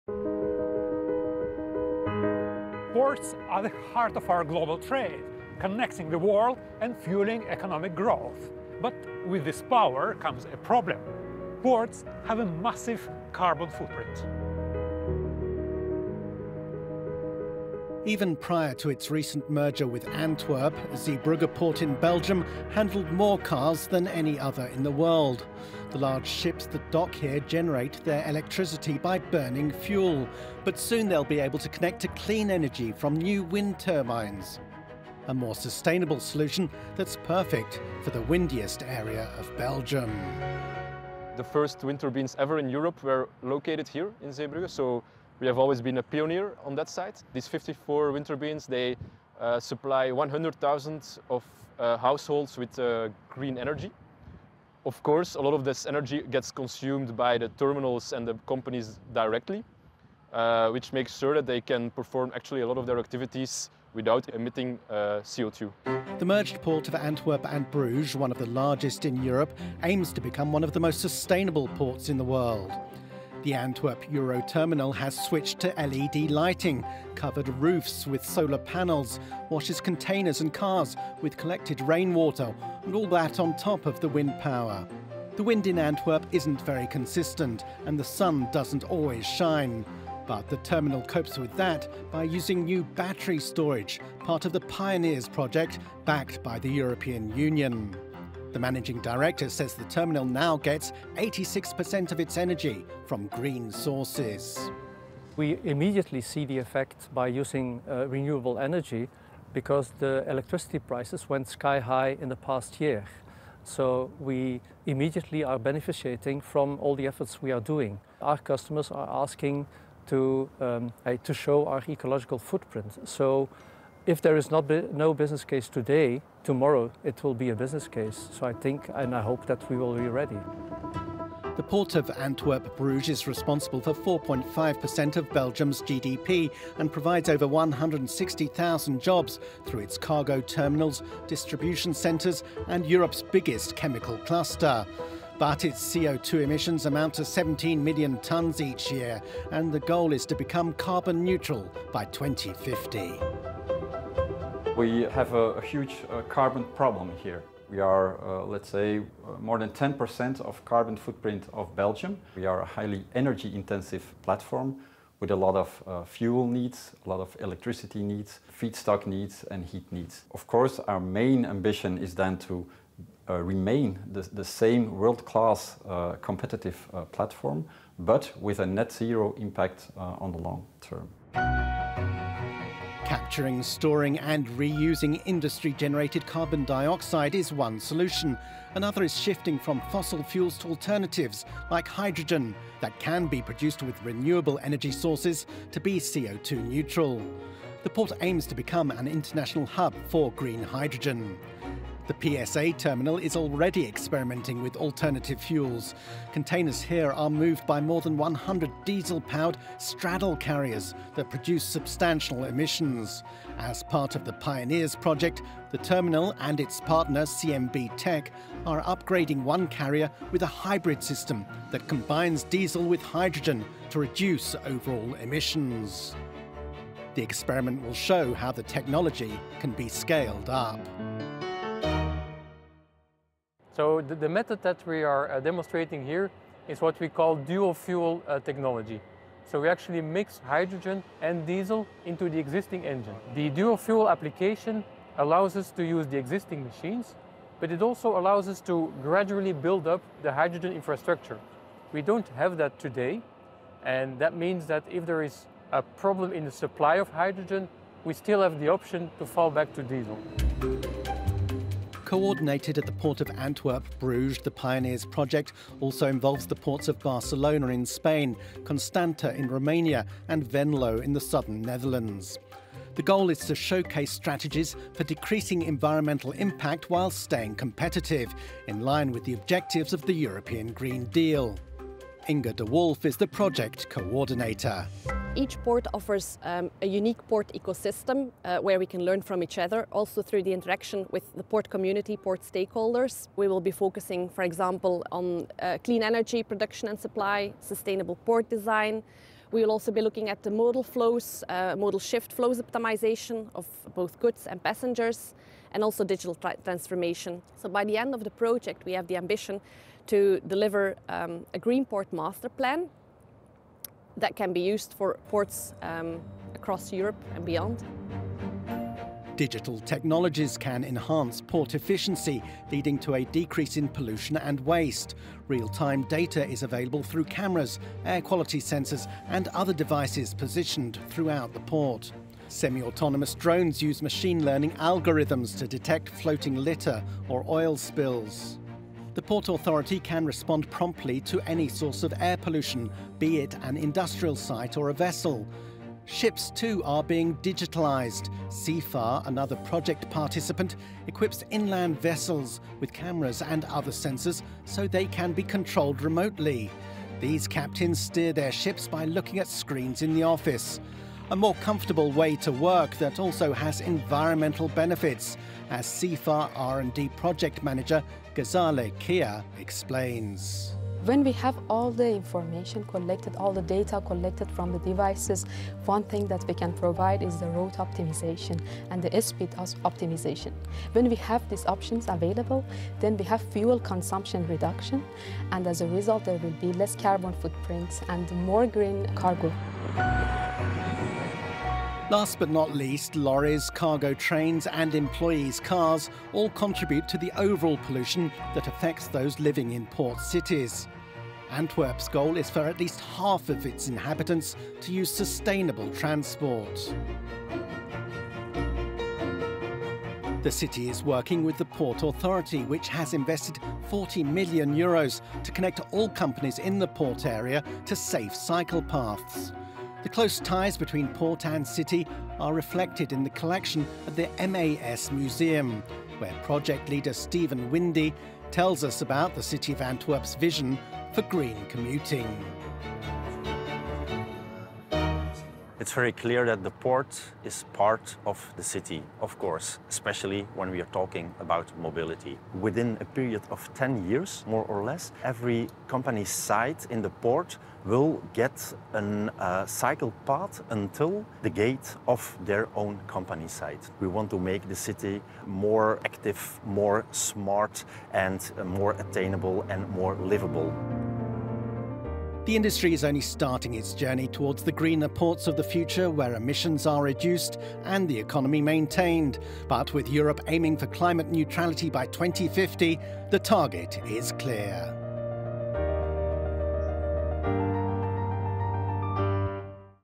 Port behind 10% of Belgium's CO₂ emissions pioneers carbon-slashing tech, aiming at net zero by 2…
A carbon footprint heavyweight, Antwerp-Bruges aims to reinvent itself as the world's most sustainable port